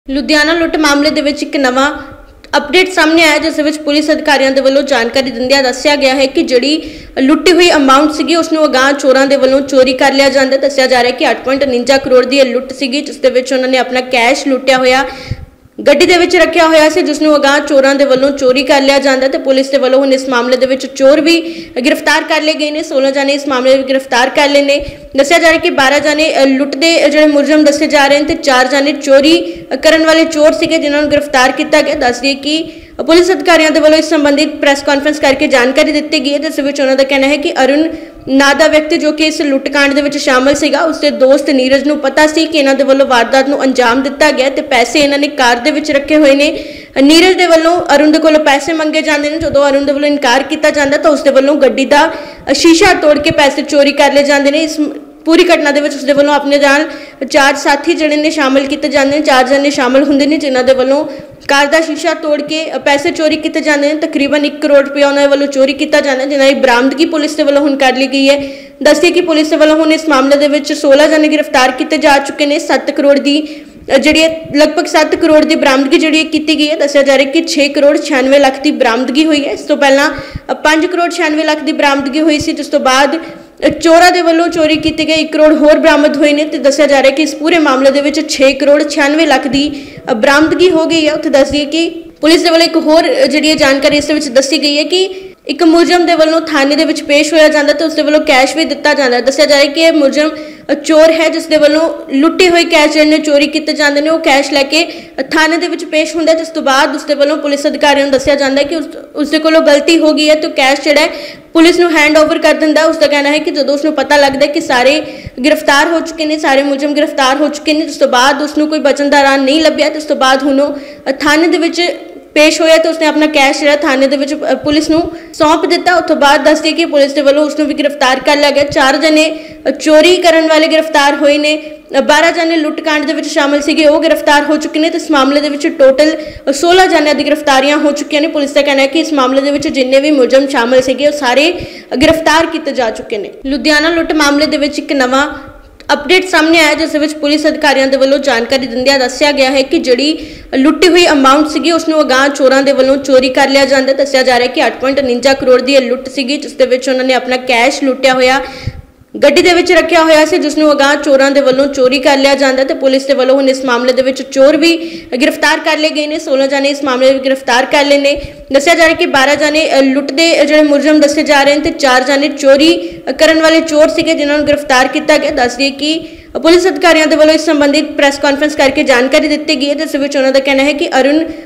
ਲੁਧਿਆਣਾ ਲੁੱਟਕਾਂਡ 'ਚ ਲੁੱਟੇ ਹੋਏ ਪੈਸਿਆਂ ਨੂੰ ਚੋਰੀ ਕਰਨ ਵਾਲਿਆਂ ਨੂੰ ਪੁਲਿਸ ਨੇ ਕੀਤਾ ਗ੍ਰਿਫ਼ਤਾਰ
Punjabi Bulletin